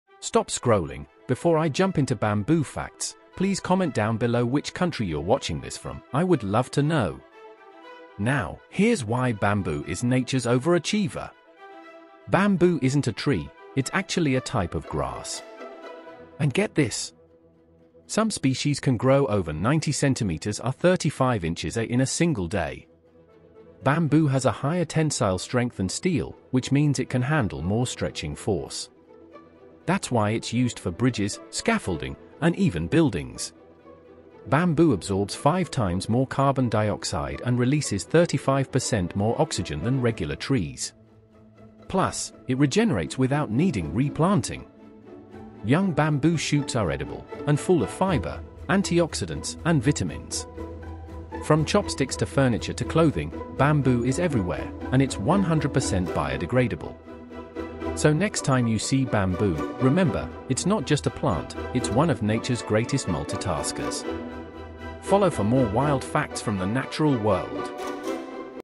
Why Bamboo Is Nature’s Most Powerful Plant
🎋 Bamboo is not a tree — it’s a super-powered grass!
It grows faster than any plant on Earth, is stronger than steel, and helps fight climate change.

In just 60 seconds, find out why bamboo might be the planet’s most underrated hero.

➕ Follow for more wild nature facts you won’t hear in school!